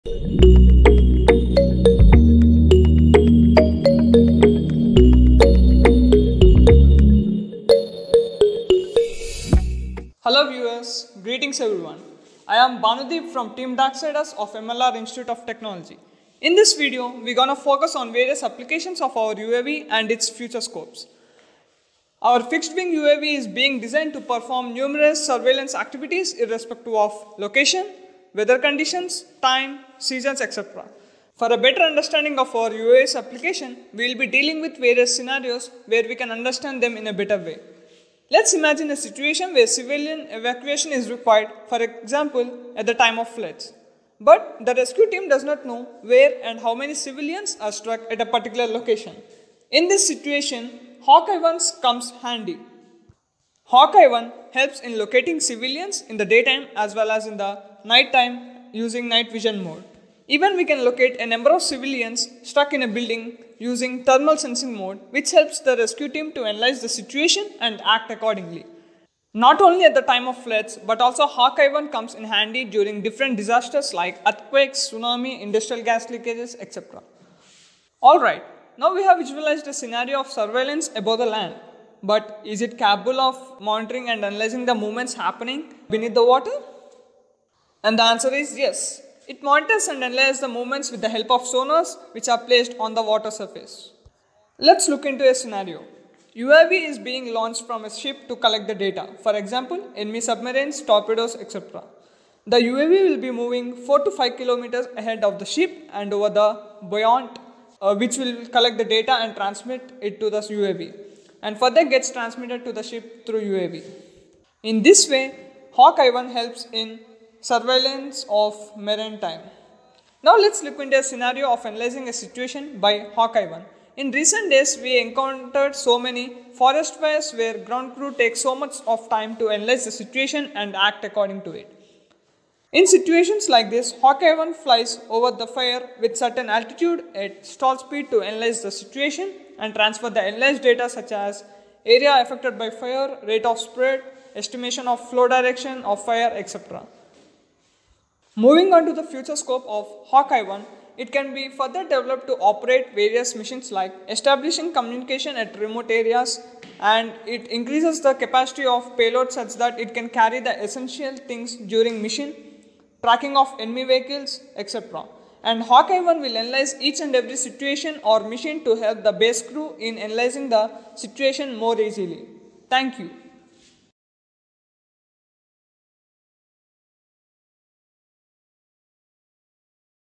HAWK-I1 || Applications of UAV || SAEADC2021 || Team DARKSIDERS || MLRIT
ADC20210115
Hawk-I1 is a UAV designed to perform various activities at different situations like disasters ,surveillance and defence with safe and easy handling. In this video we gonna show applications of HAWK-I1 during various circumstances. This UAV is being designed by the team DARKSIDERS from MLR Institute of Technology. To get more updates on HAWK-I1 and other Aeronautical related content